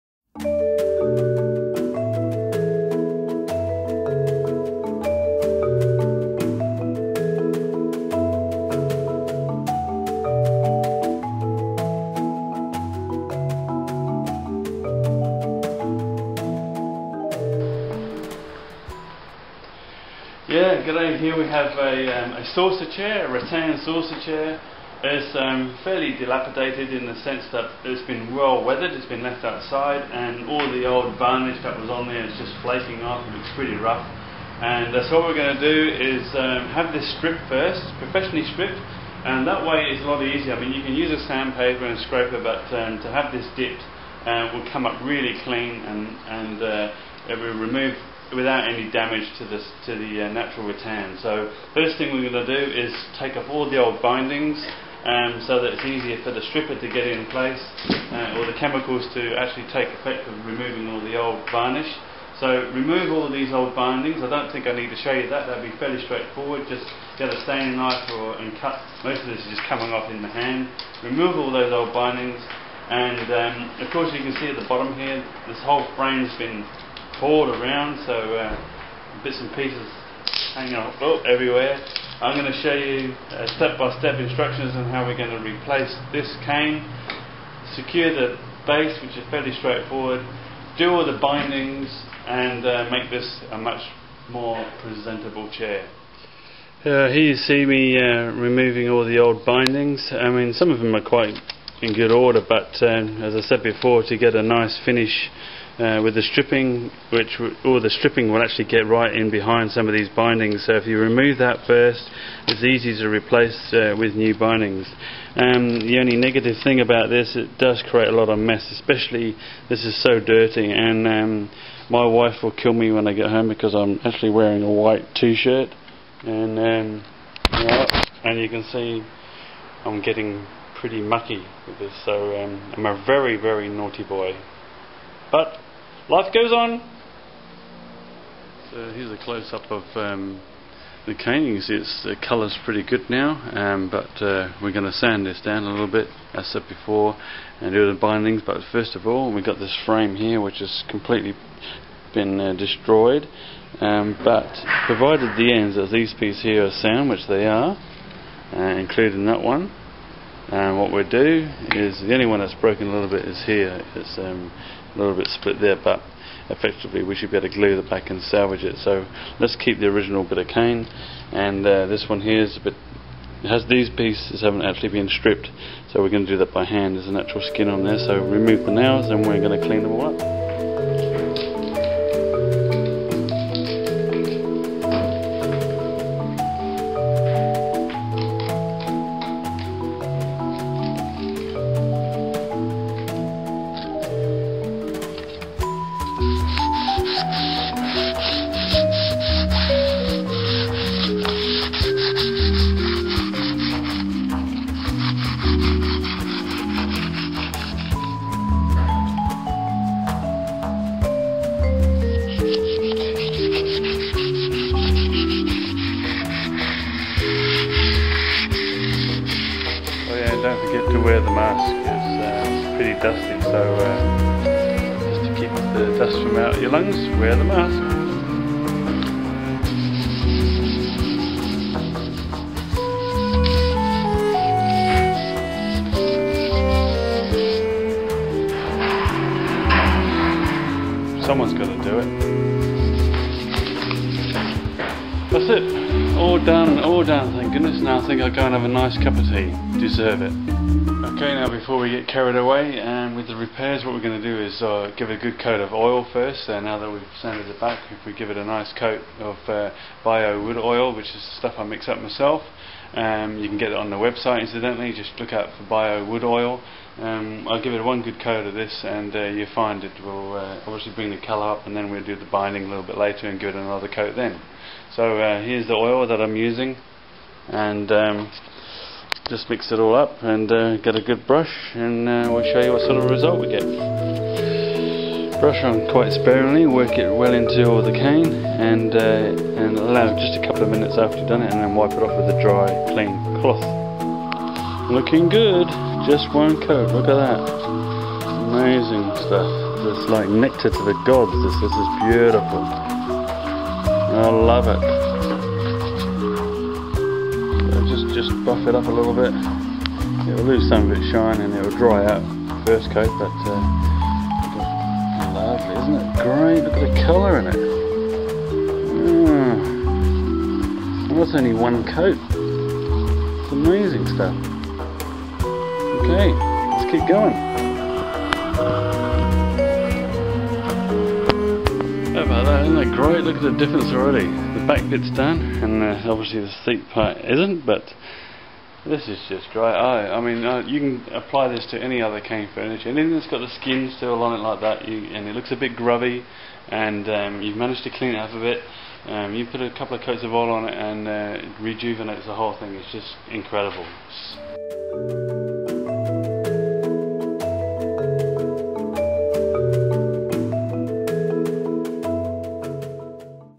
How To Restore a Cane Saucer Chair Pt.1.
Easy Step by Step Instructions on How to Restore a Cane Saucer chair Pt.1